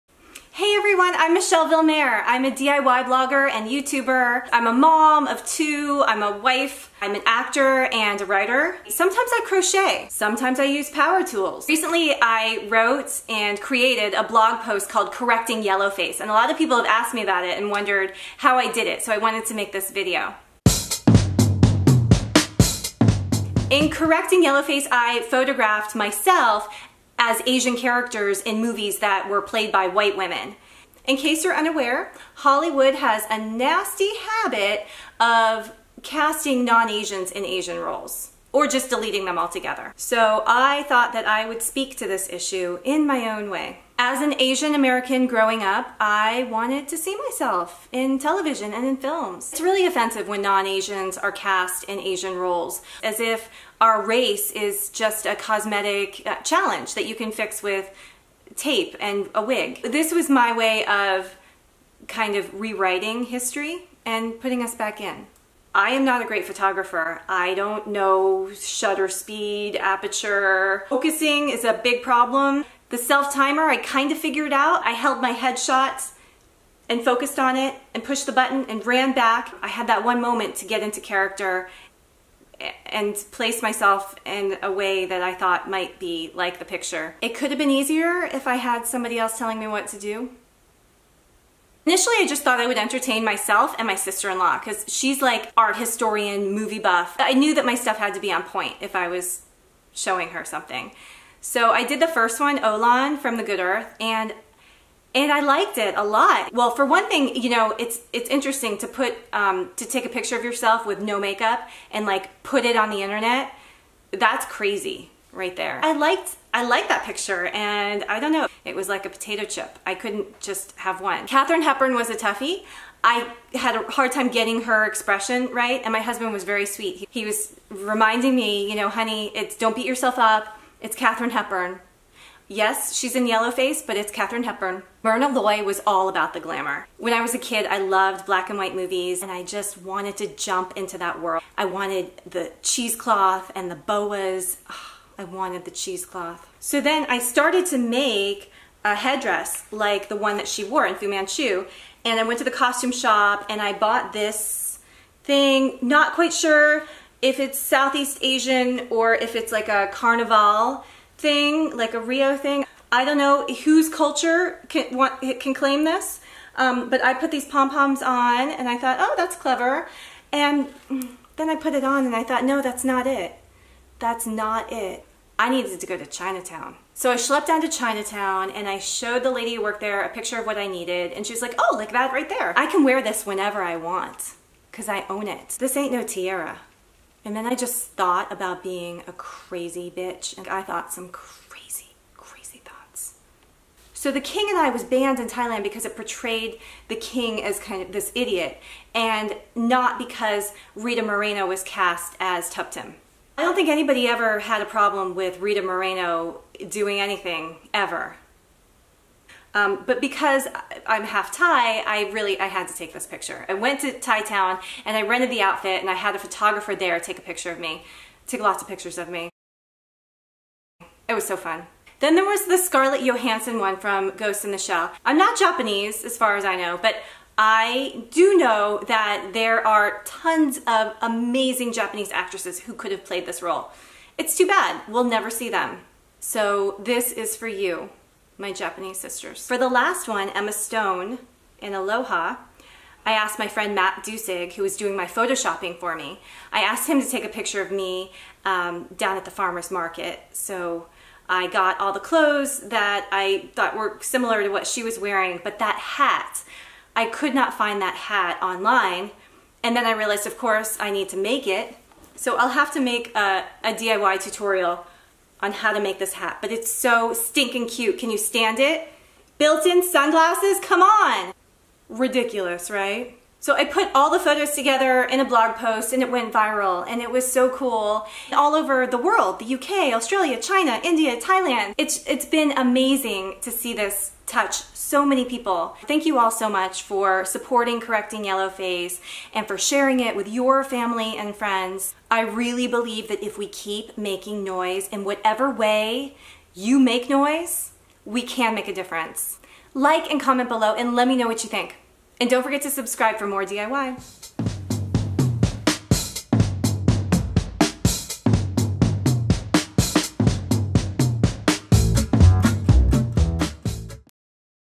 Correcting Yellowface
Correcting Yellowface, my DIY response to the Hollywood whitewashing of Asians.